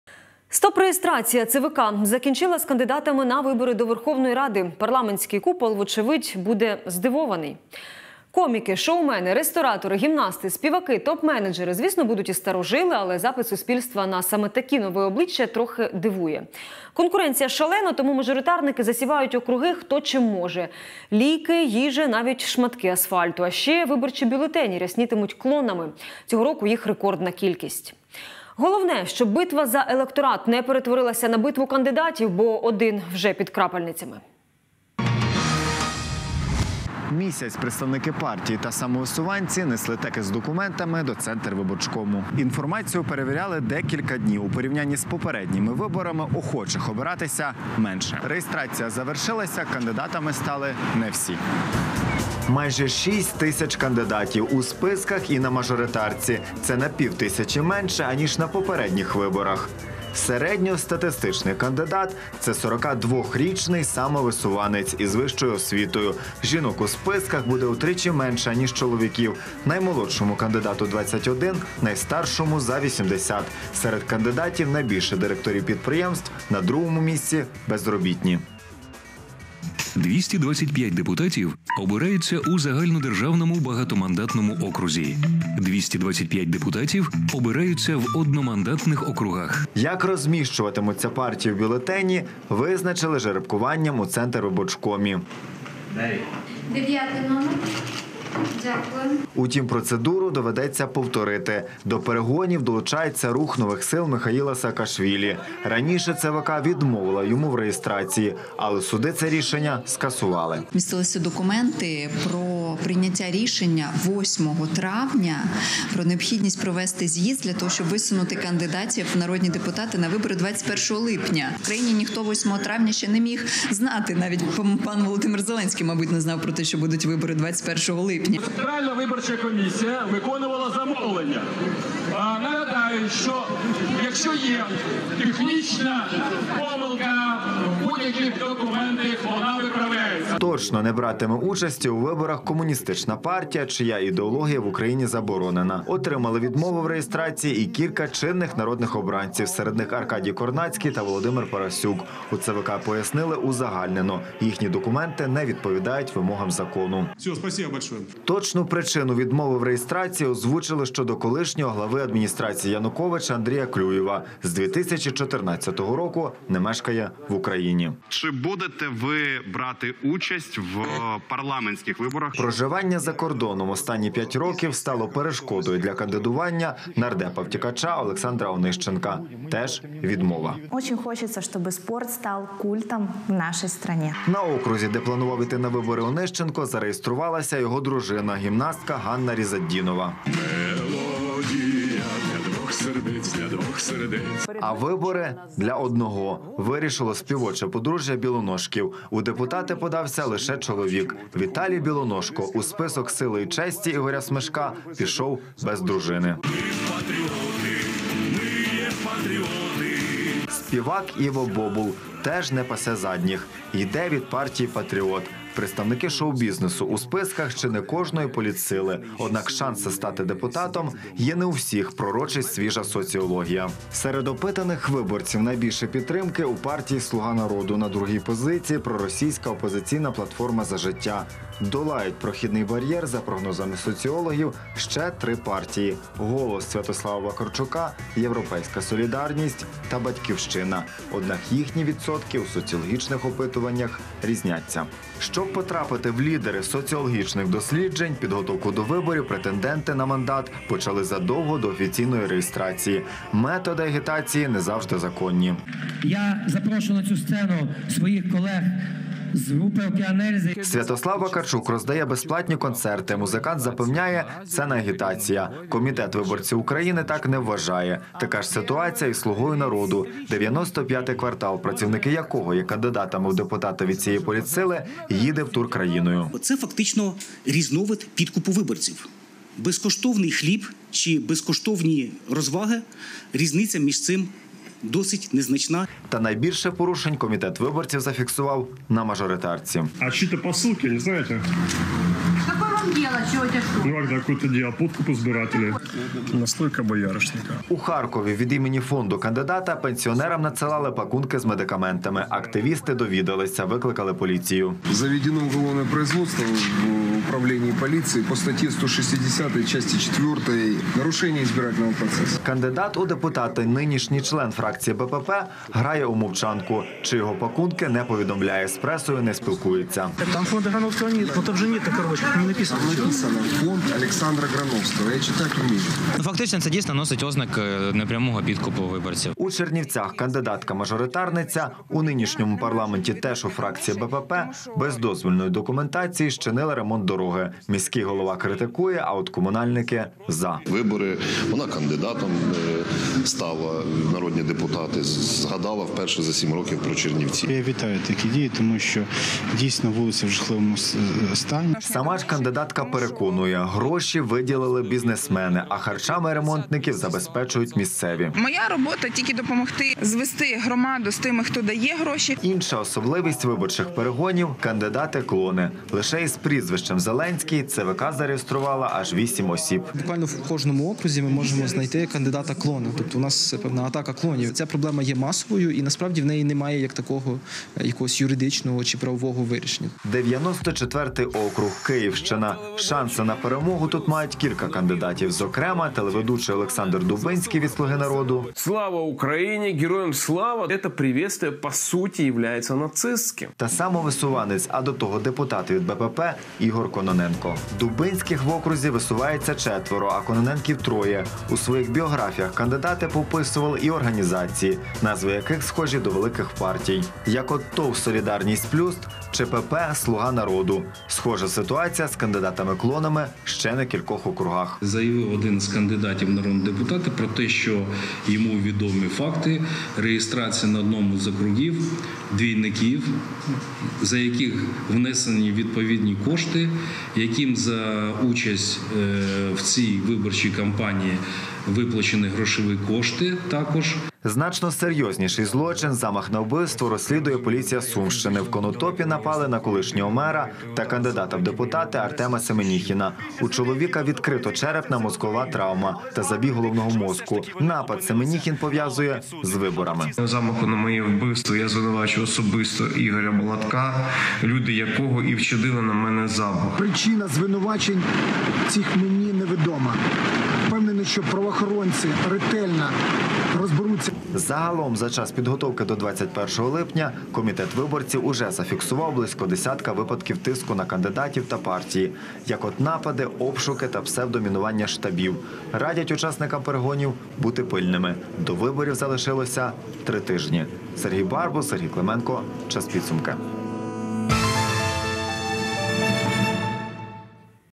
Стоп - реєстрація: скільки набралося кандидатів і як вони заграють з виборцями
ЦВК закінчила з кандидатами на вибори до Верховної Ради. Парламентський купол вочевидь буде здивований. Коміки, шоумени, ресторатори, гімнасти, співаки, топ-менеджери. Звісно будуть і старожили, але запит суспільства на саме такі нові обличчя  - трохи дивує. Конкуренція шалена, тому мажоритарники засівають округи хто чим може. Ліки, їжа, шматки асфальту. А ще виборчі бюлетені ряснітимуть клонами. Цього року їх рекордна кількість. Головне щоб битва за електорат не перетворилась на битву кандидатів. Бо один вже під крапельницями.